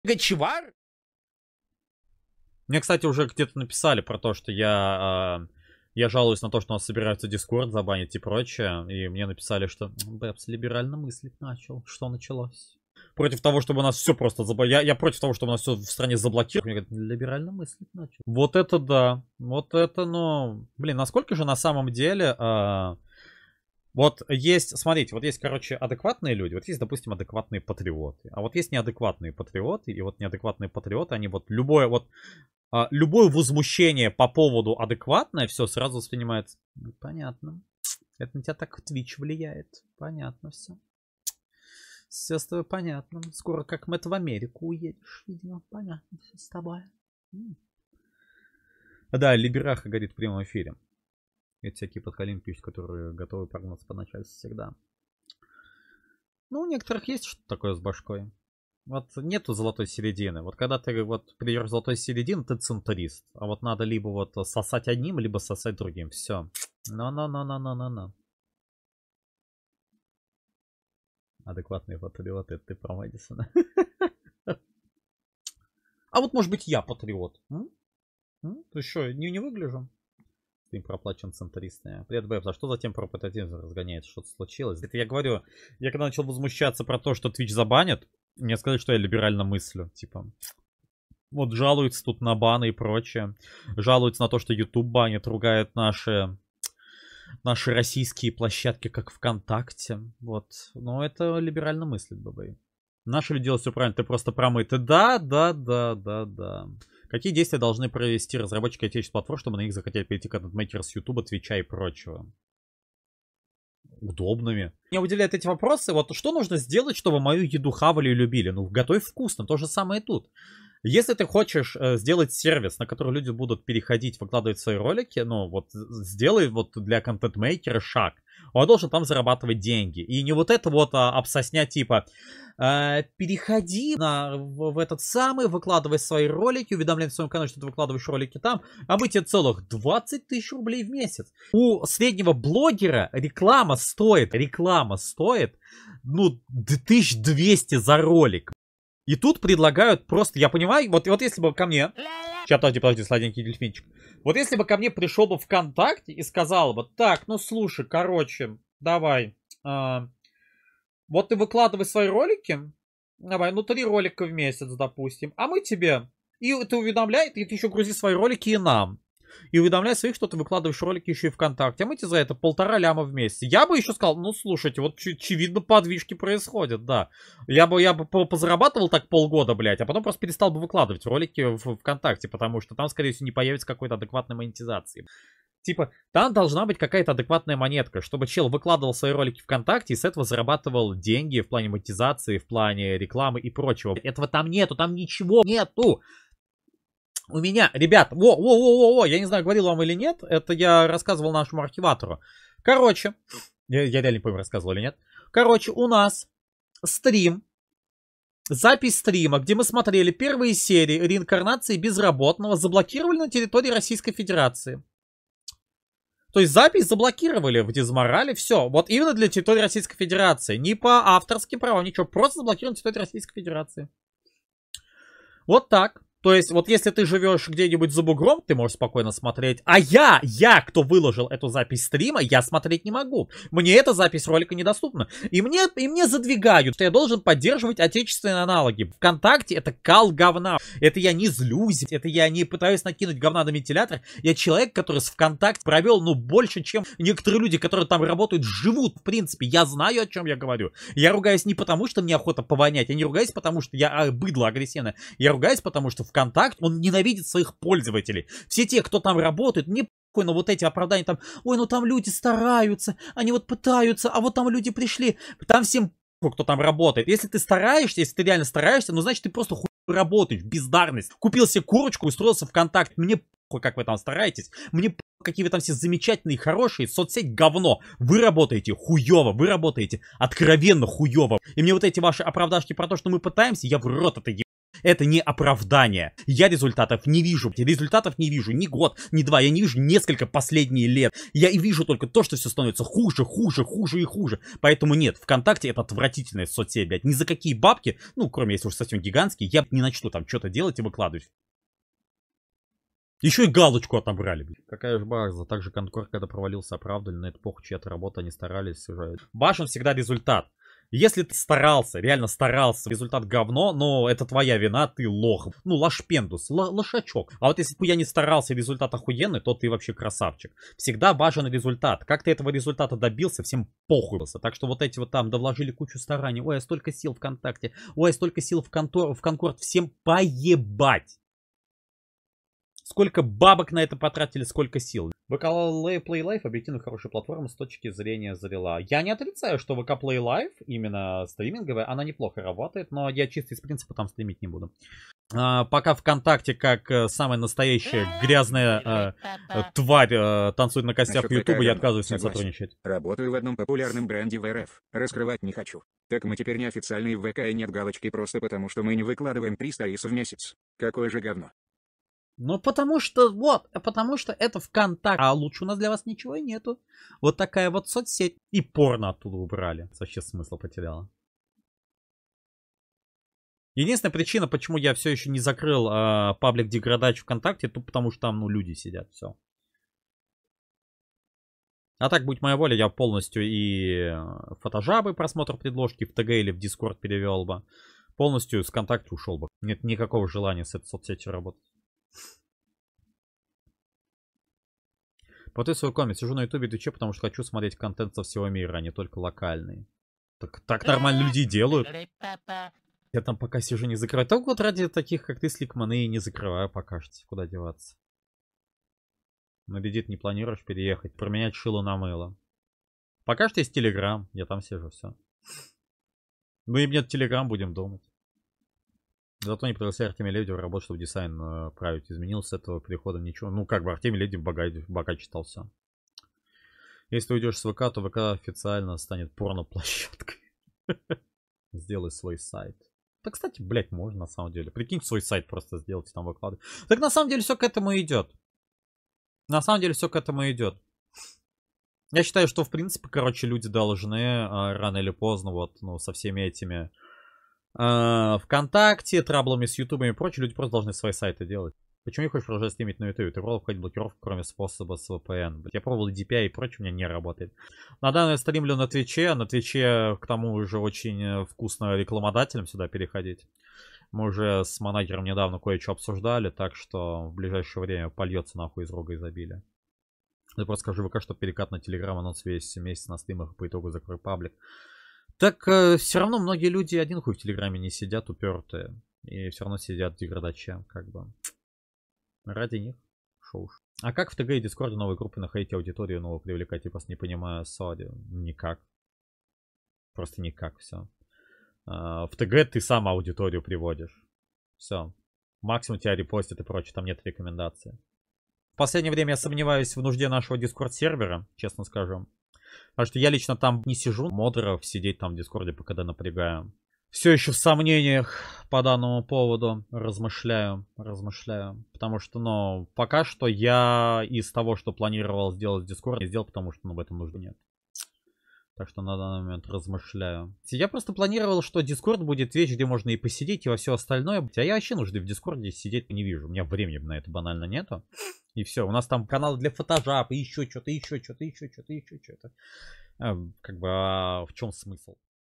Бэбэй ЖЁСТКО про БЛОКИРОВКУ ДИСКОРДА и РОССИЙСКИЕ ПЛАТФОРМЫ
В этой нарезке Бэбэй высказался про бан дискорда, а так же обсудил перспективы развития российских платформ для контентмейкеров